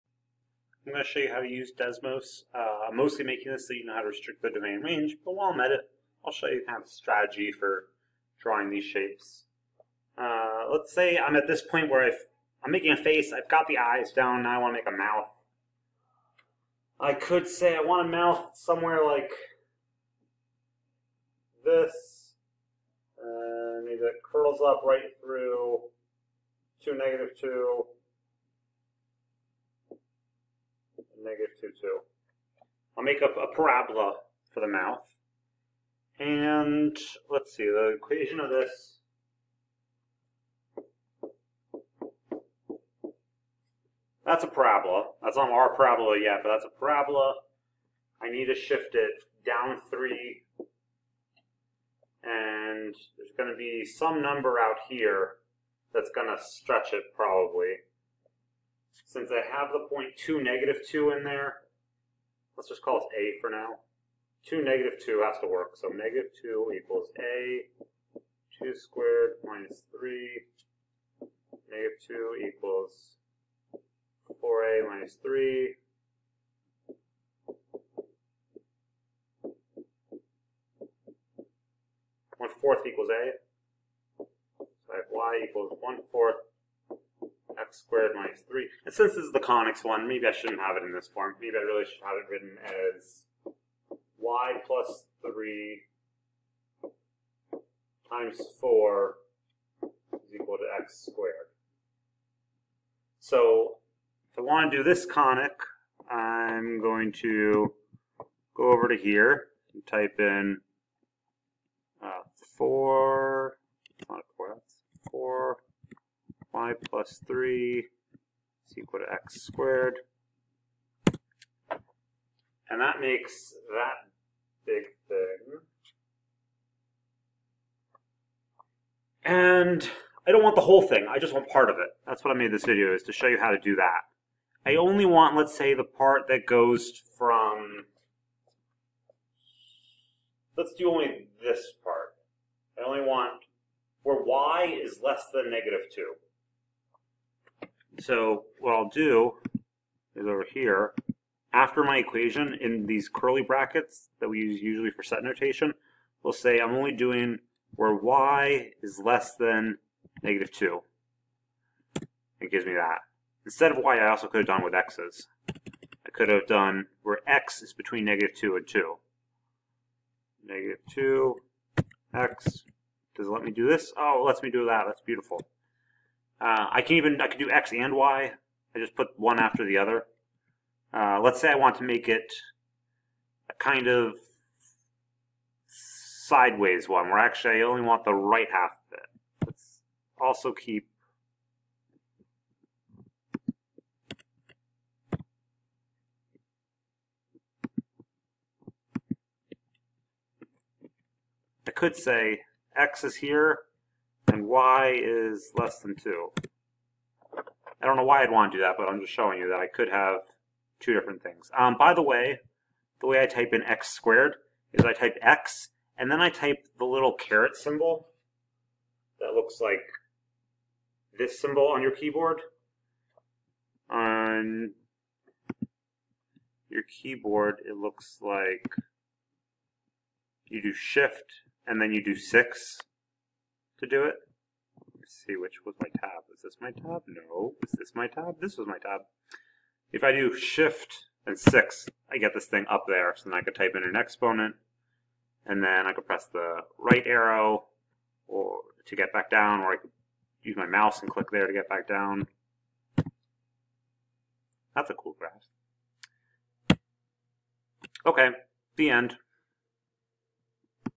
How To Restrict Domain and Range on Desmos
An explanation of how to use desmos.com to graph equations with restricted domains or ranges. (video created for my students, but maybe it'll be helpful to strangers as well...)